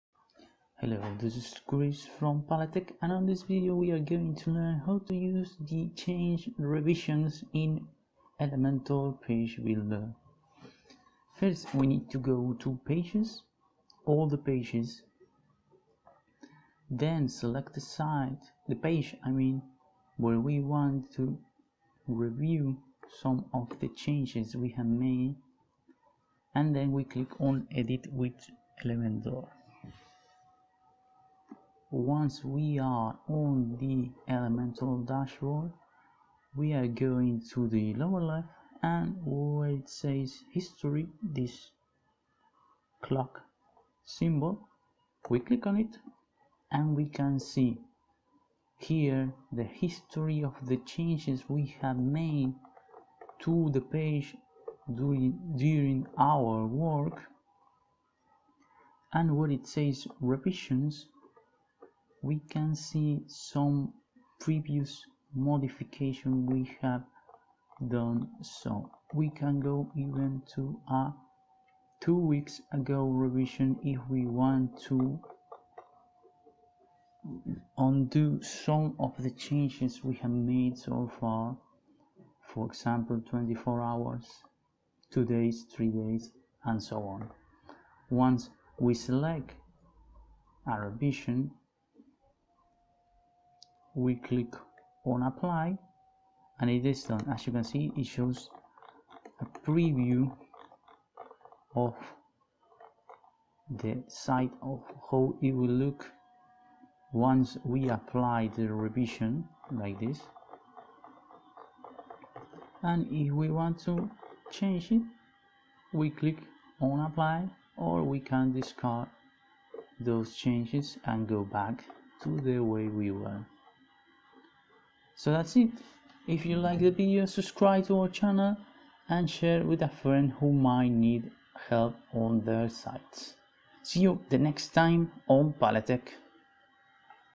How to use Revisions in Elementor
Easy way to undo or redo changes in Elementor Page builder plugin.